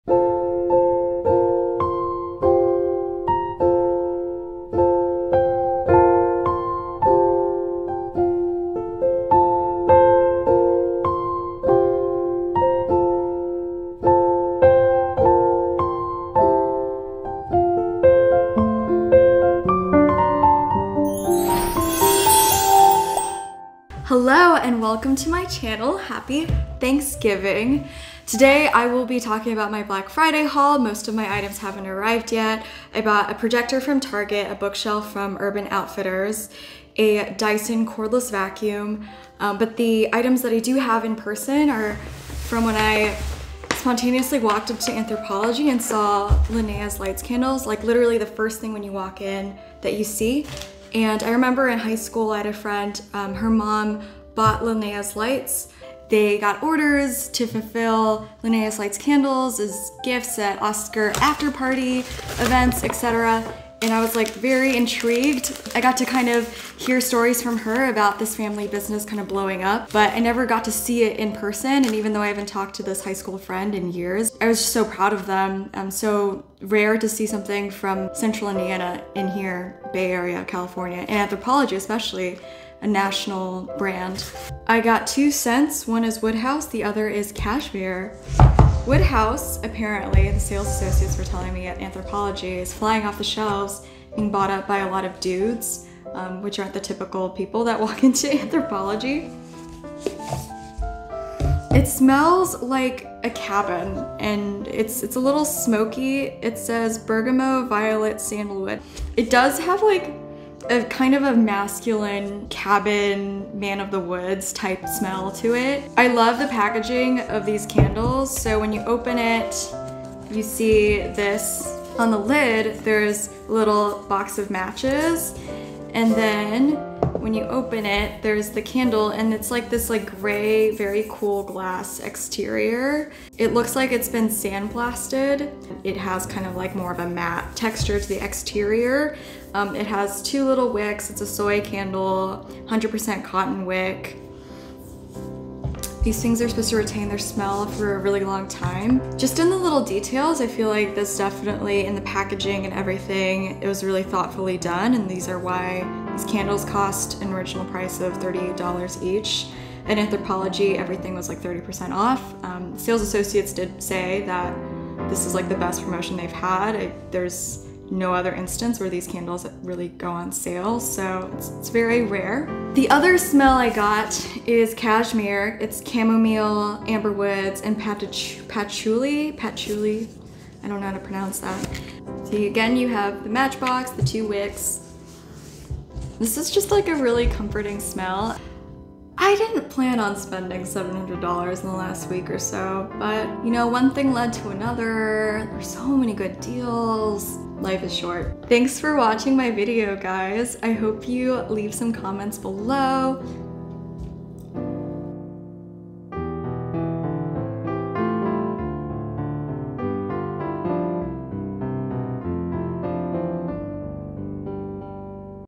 Linnea's Lights Candles Unboxing | Splurging on Scents BlackFriday Anthropologie Vlog
Sorry, it took a while to edit, so I got this video out a week after Thanksgiving. I may do another video later where I actually light the candles and smell them.

0:00 Intro
1:29 Wood House Candle
2:38 Pricing/Cost/Promotion for Black Friday
2:54 Cashmere

Intro was made using Vimeo Create